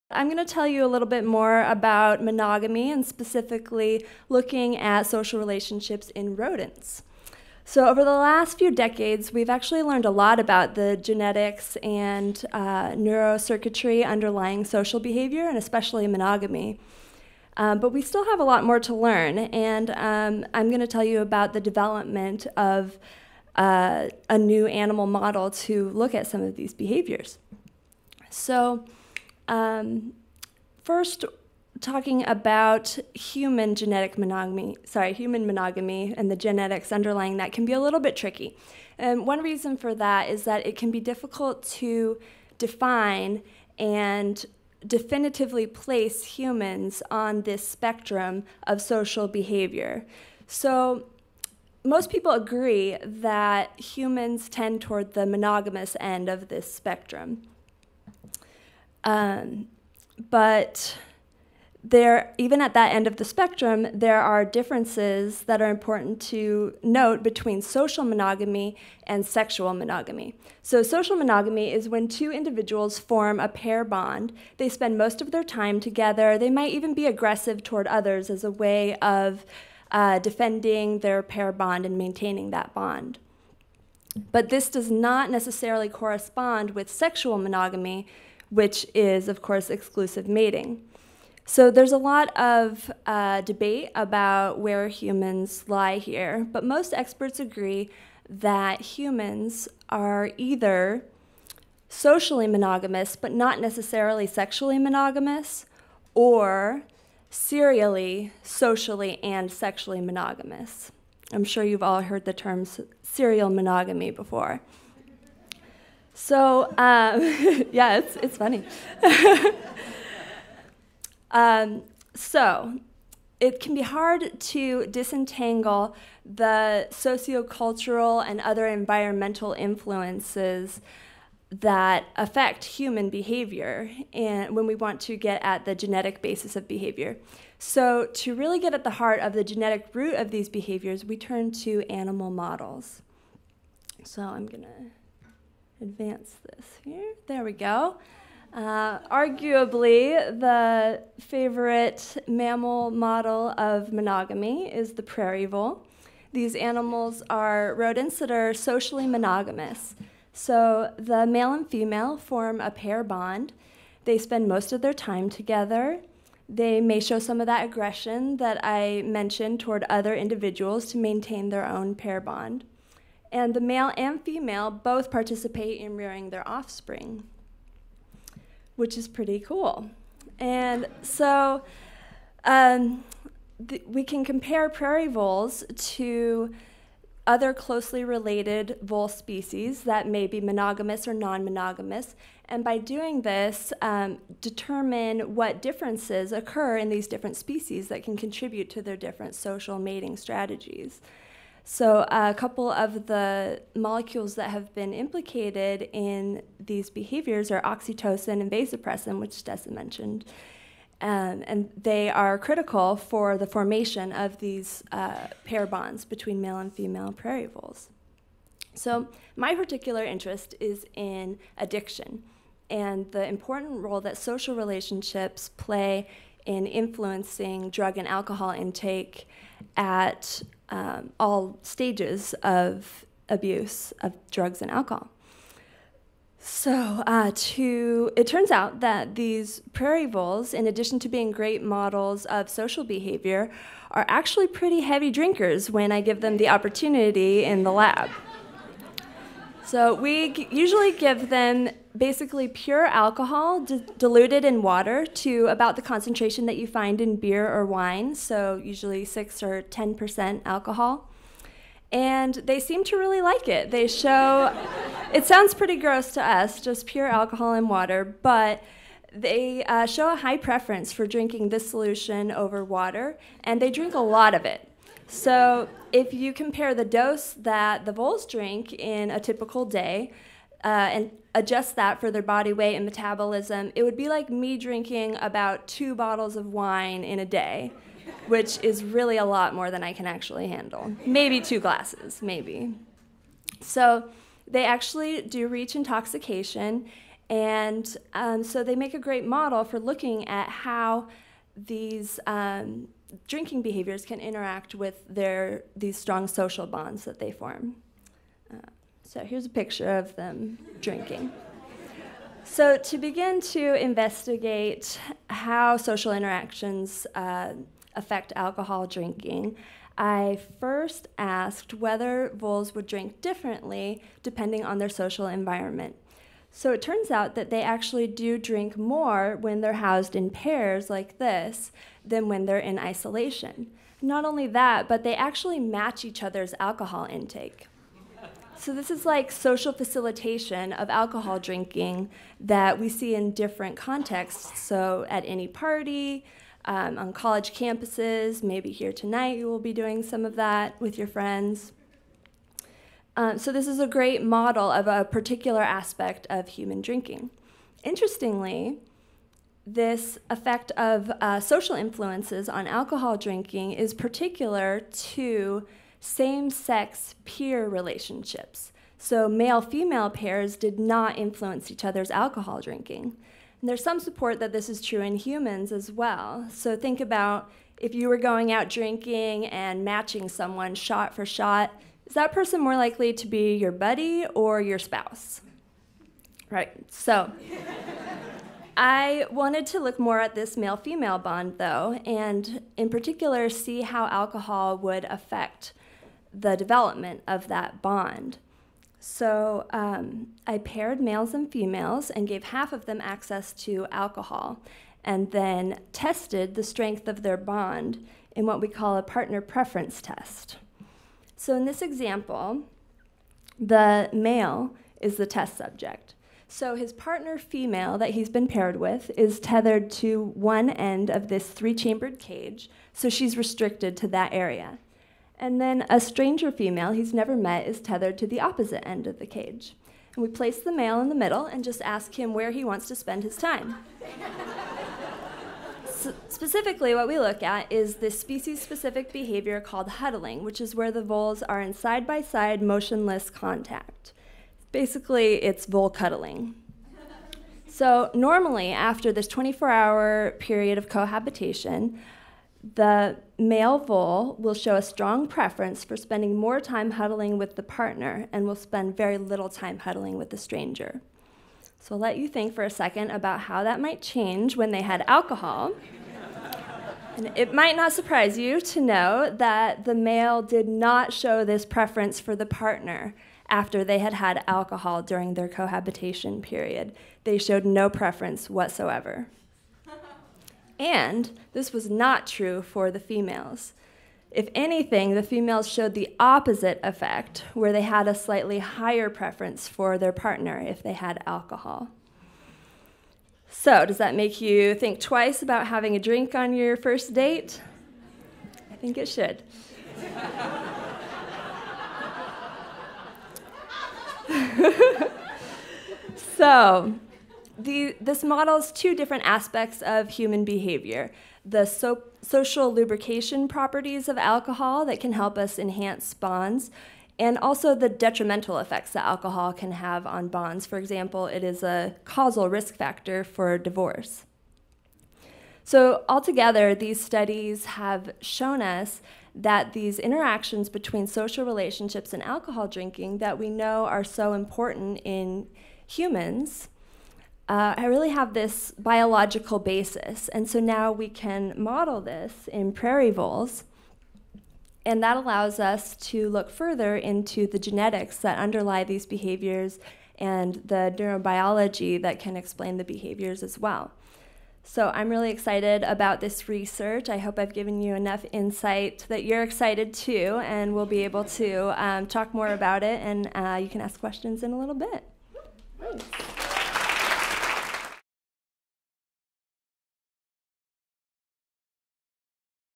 Heartbreakers: What Can Voles Teach Us About Monogamy?
Could our genes affect our romantic choices? Does love inhabit a particular region of the brain? If this song makes us too sad to drive straight, why are we playing it on repeat?

Rapper and essayist Dessa hosts "Heartbreakers" - a four-event collision of art, music and scientific research on love, sex and human adventures in pair-bonding.

Behavioral neuroscientist Allison Anacker joined us to explain how research on voles might reveal a genetic basis for monogamy in humans, and how booze affects it.